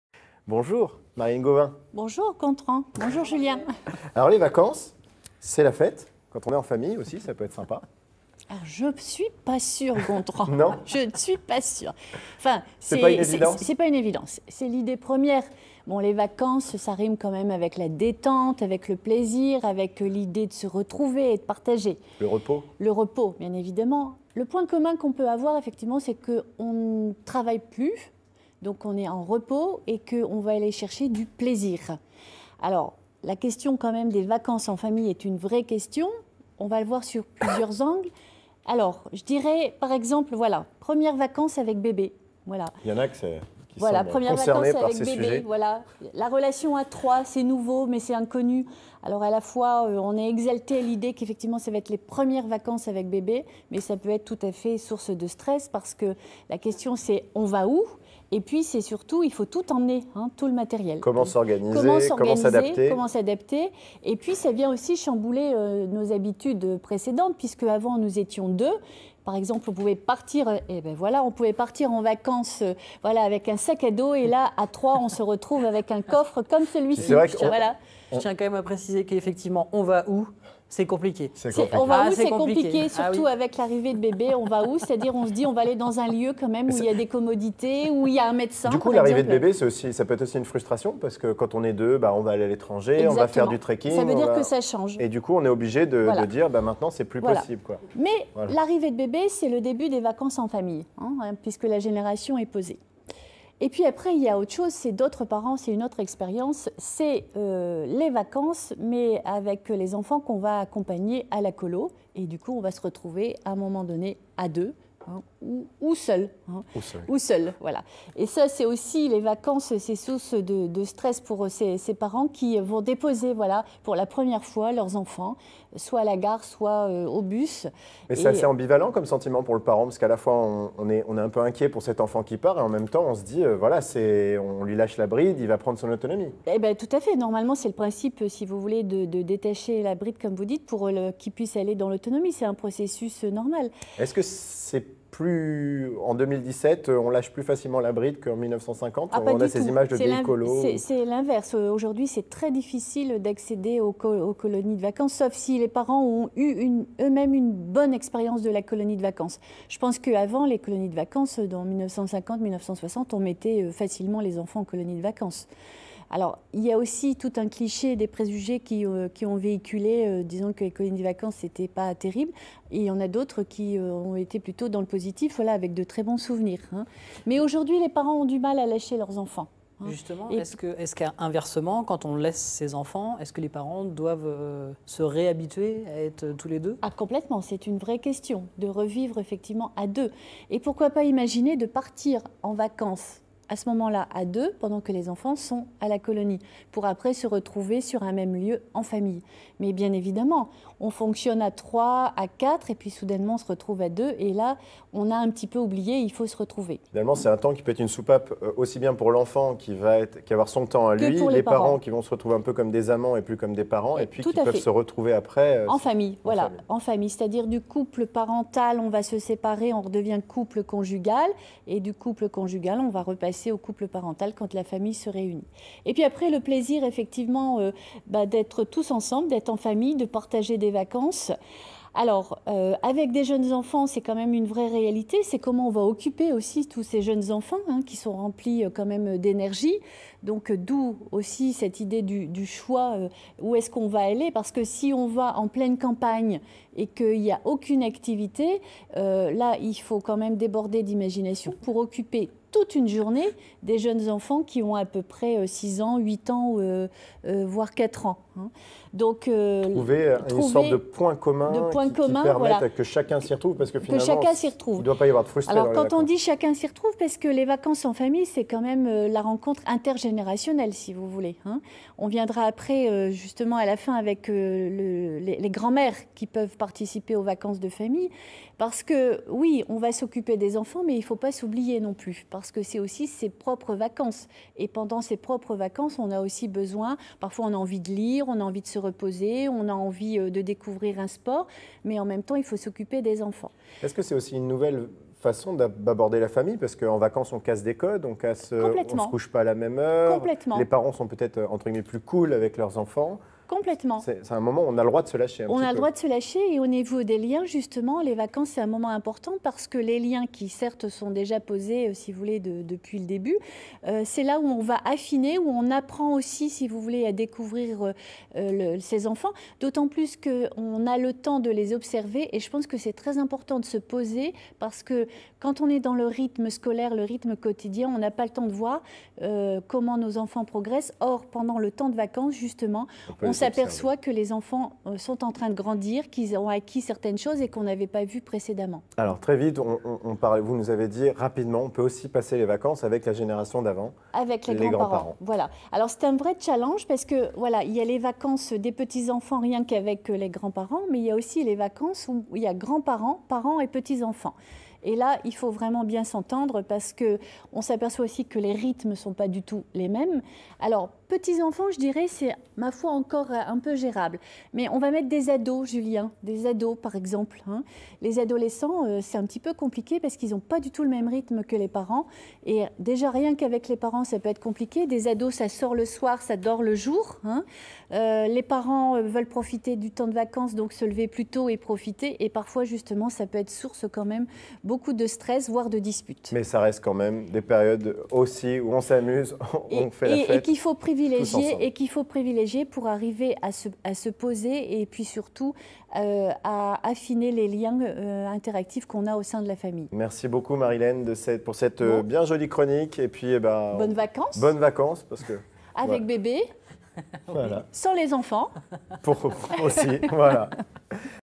Vacances en famille : un bienfait ou une source d'angoisses ?
En famille, les vacances peuvent rimer avec plaisir, détente, partage mais aussi avec stress, à l’idée de les préparer, de se séparer ou au contraire, de se retrouver tous ensemble réunis.
La psychologue Marylène Govin nous aide à faire les bagages en parant à toutes les éventualités.